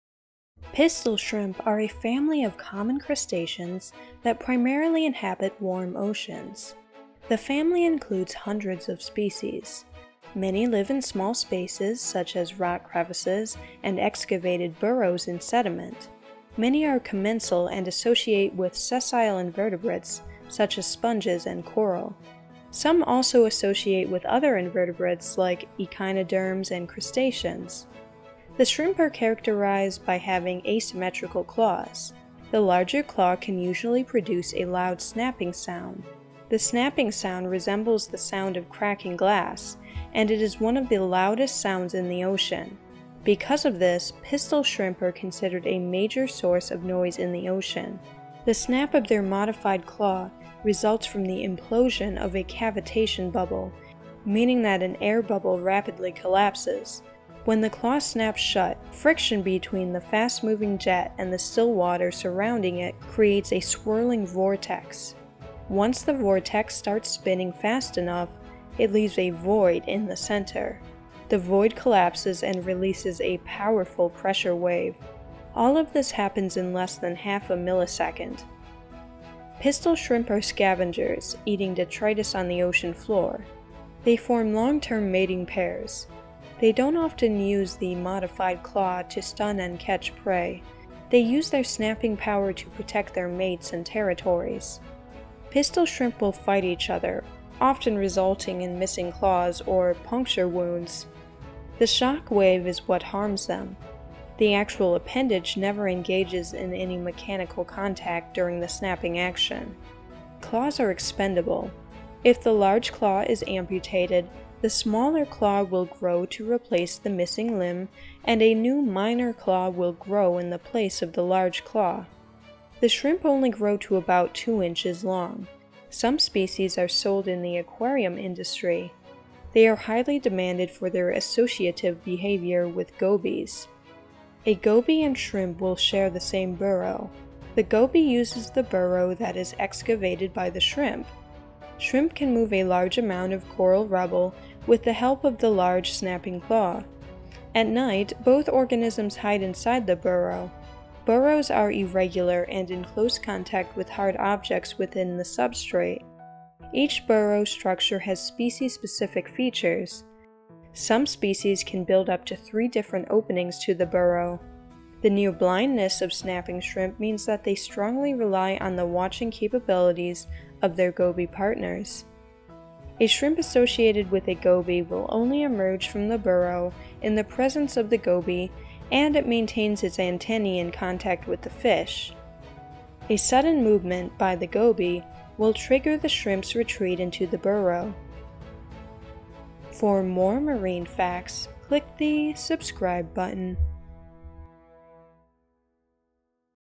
Facts: The Pistol Shrimp
Quick facts about one of the loudest animals in the ocean! The pistol shrimp (snapping shrimp, Alpheidae). Pistol shrimp facts!

Calado, Ricardo. Marine Ornamental Shrimp: Biology, Aquaculture and Conservation. Germany, Wiley, 2009.
By Laura FLAMME - MNHN - Museum national d&#039;Histoire naturelle (2020). The crustaceans collection (IU) of the Muséum national d&#039;Histoire naturelle (MNHN - Paris). Version 68.158. Occurrence dataset accessed via GBIF.org on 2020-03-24.
By The original uploader was Hkchan123 at Chinese Wikipedia.
By Anker A, Grave S - Anker A, Grave S (2012). &quot;Description of Alpheus cedrici sp. n., a strikingly coloured snapping shrimp (Crustacea, Decapoda, Alpheidae) from Ascension Island, central Atlantic Ocean&quot;. ZooKeys 183: 1--15. Pensoft Publishers.